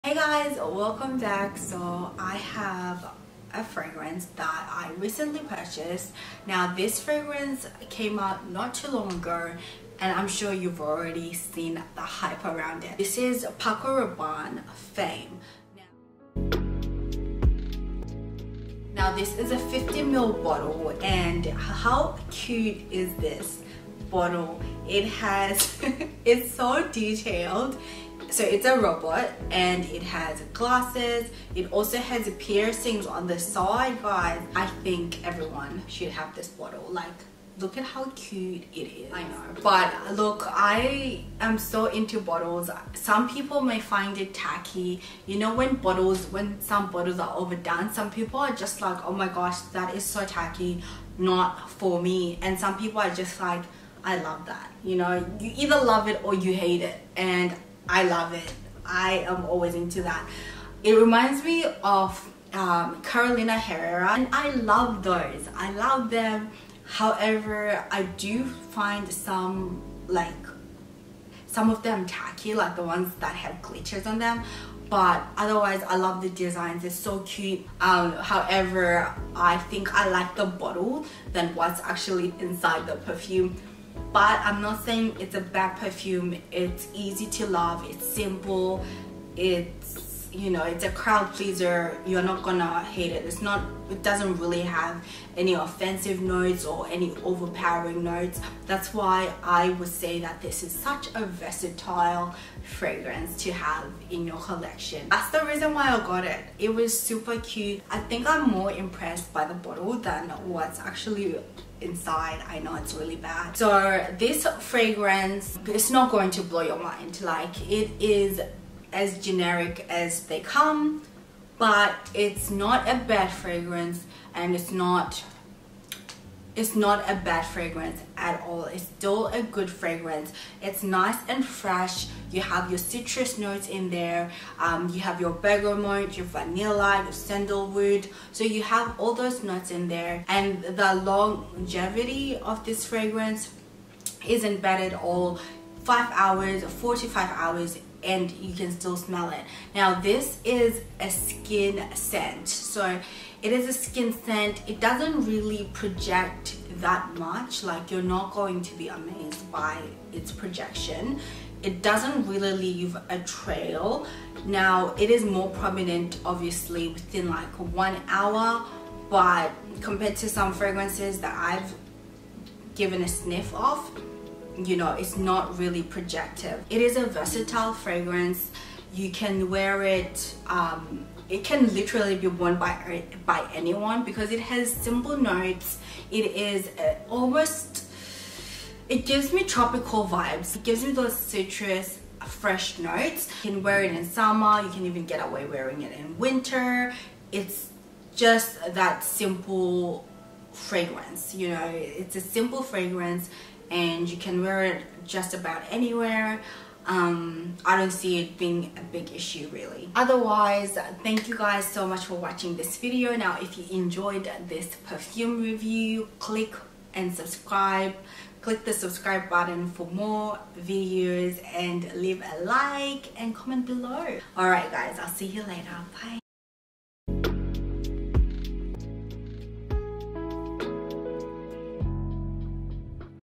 PACO RABANNE FAME PERFUME REVIEW
Review of Paco Rabanne's fame

Hope you enjoy watching x

Socials:
TikTok Gwebbu